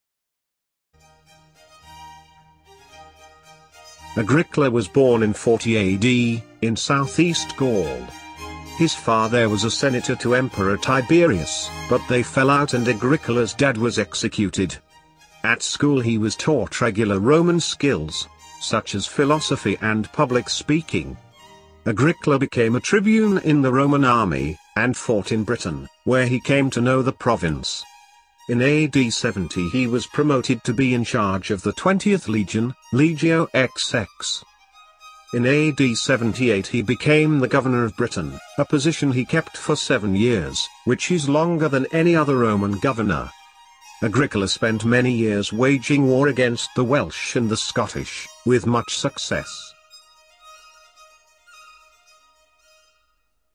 Agricola, governor of Britain
This is the general story of Argricola a famous figure in the Roman world.
Brit Flyer ©